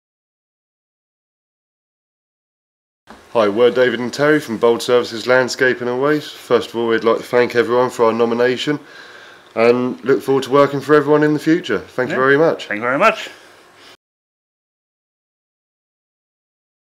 South England Prestige Awards 2022/23 Winner
We invited winners from our South England Prestige Awards 2022/23 to Oakley Hall Hotel on the 2nd & 3rd March 2023 to meet the judges and celebrate their Win.